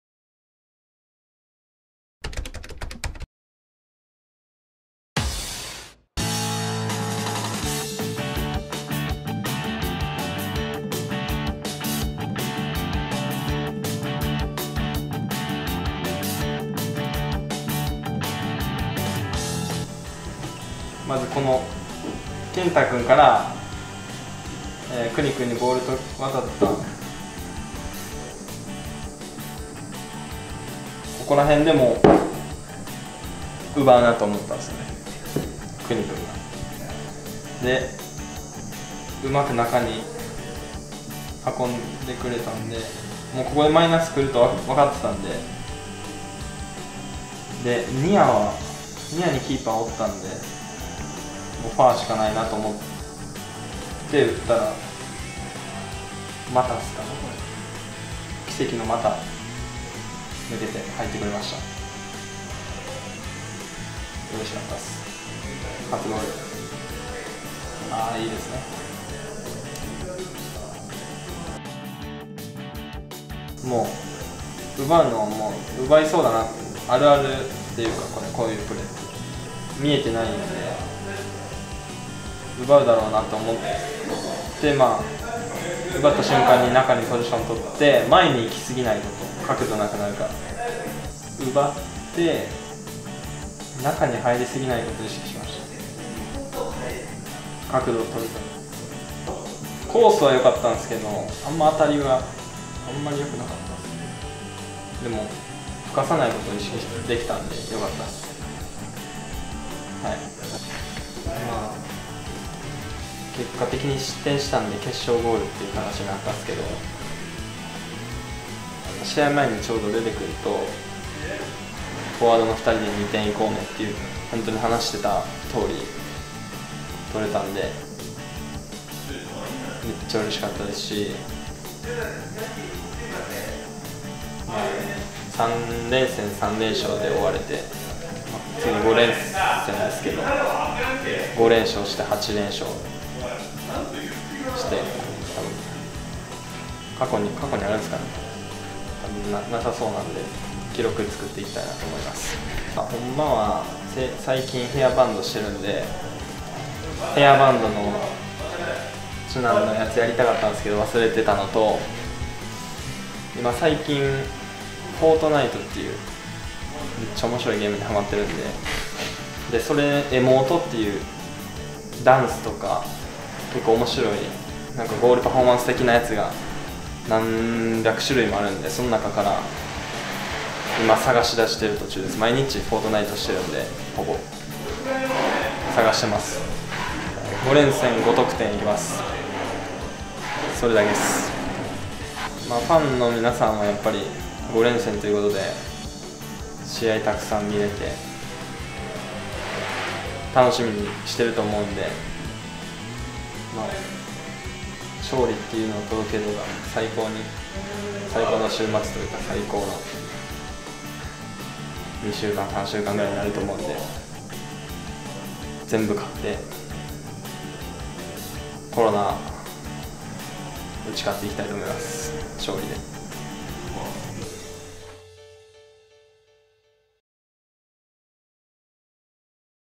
PLAYER's CHANNEL ~第9節松本戦 町野修斗選手 J２初ゴール~
選手自身が試合のプレーを振り返ります！
今回は第9節松本戦での決勝点となった町野修斗選手の J２初ゴールをピックアップ！